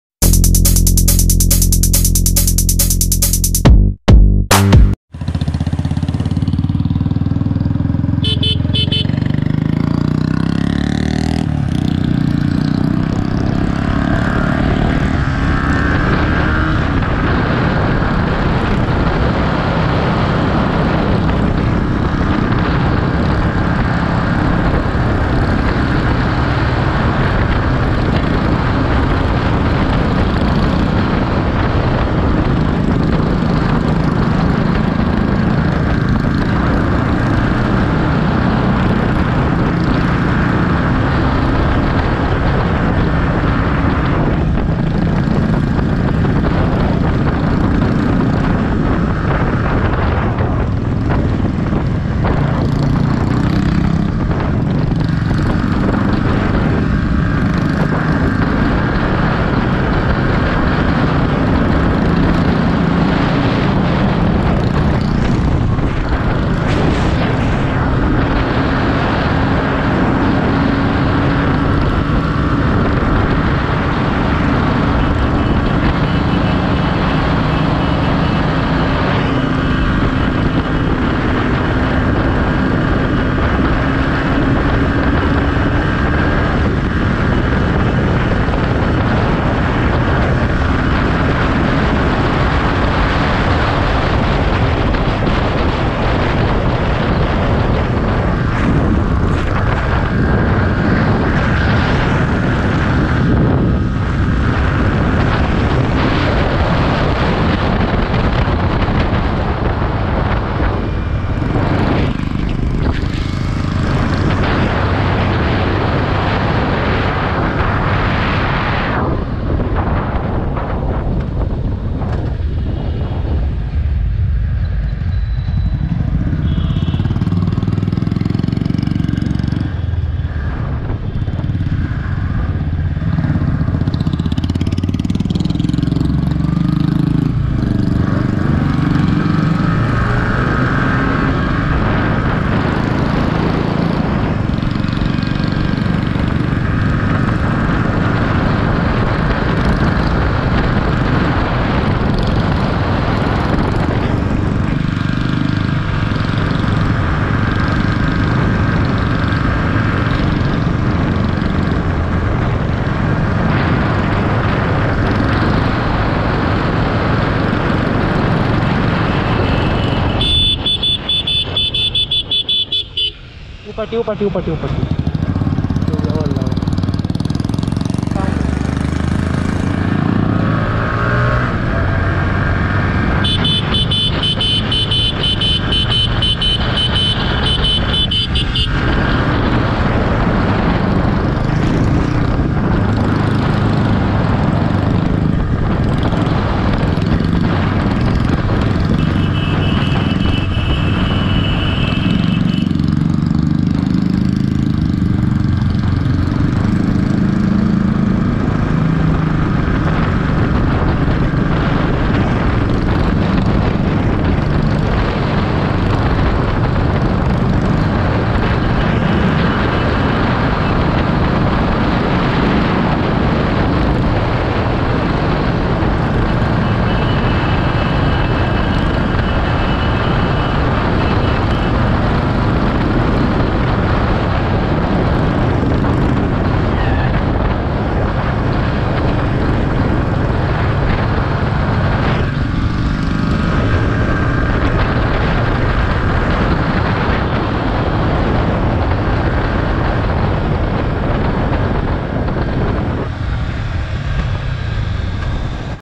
Bullet Crashed?!😱 ll Crazy Bullet Rider V/s R15 V3 BS6
i m a motovlogger from patna show your support and love